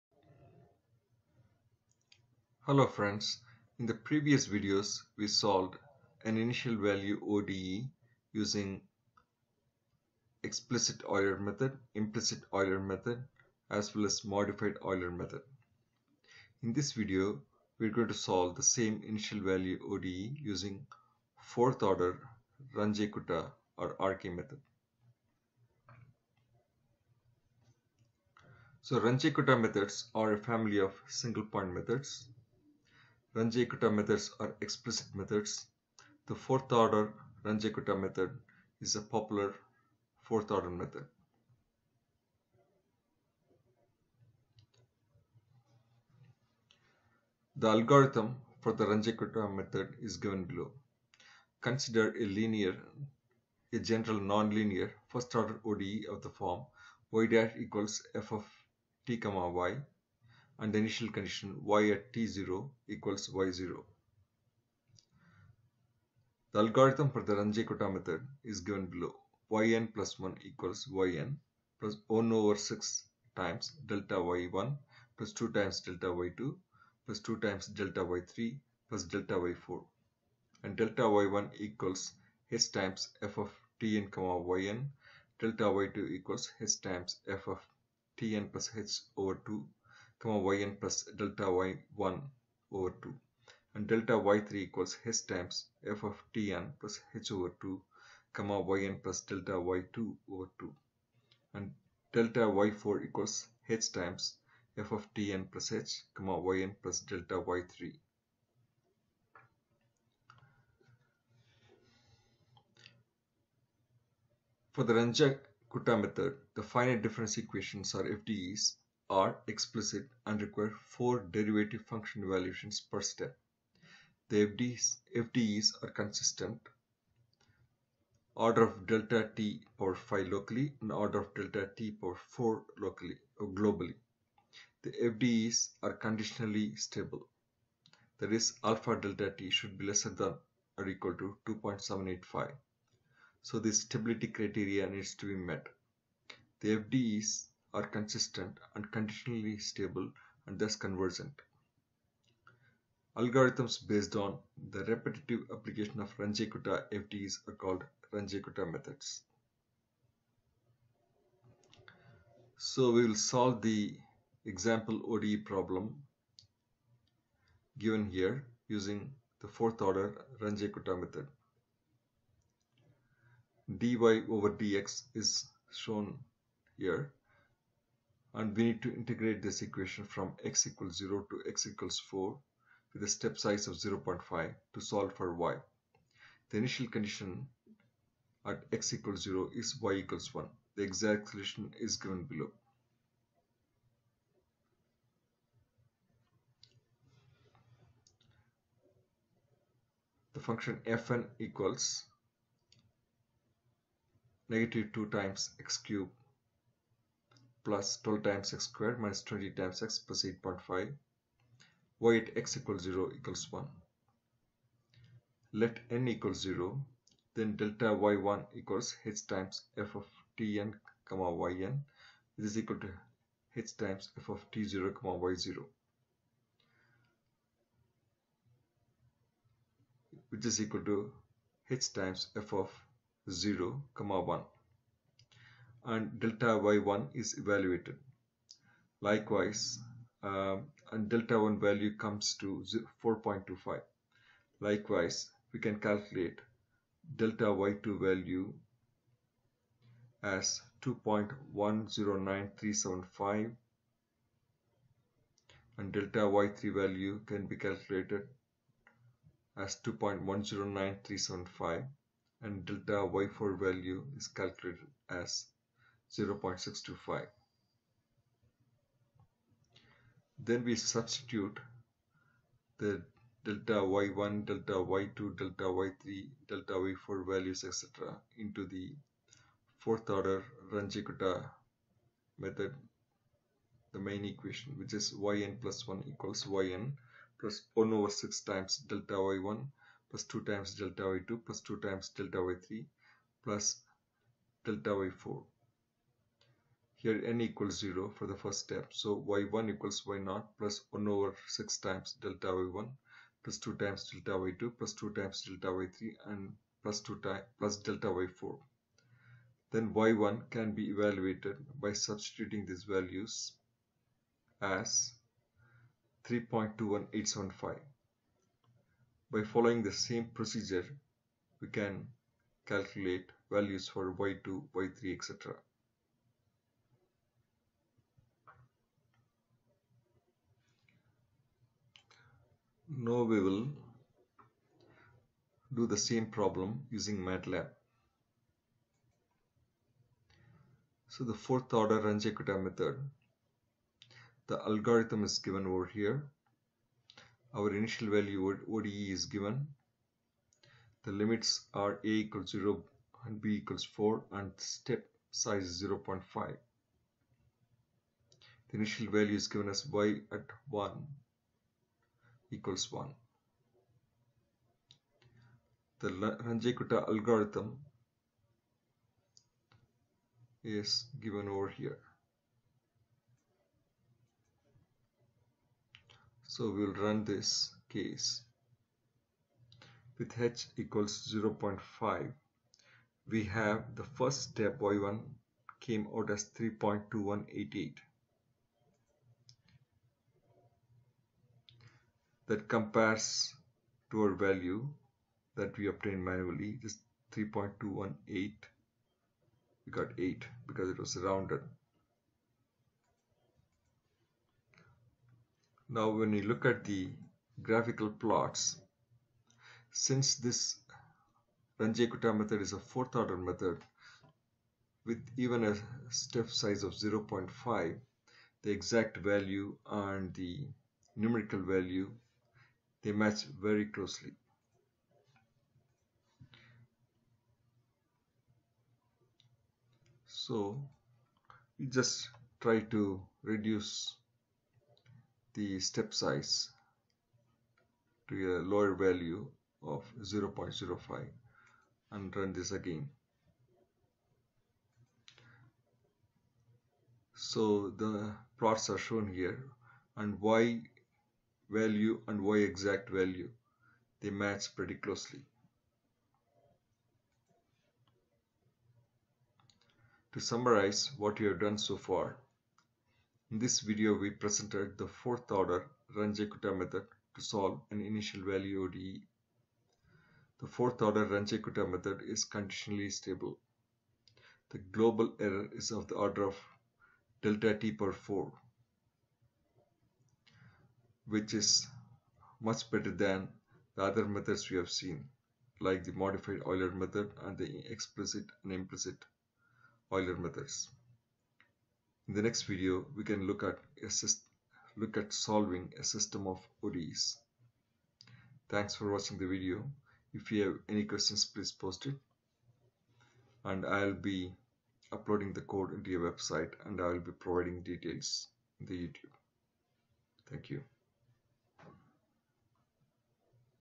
Solve Initial Value ODE Using Fourth-Order Runge-Kutta Method
Use the Fourth-Order Runge-Kutta Method to solve Initial Value ODEs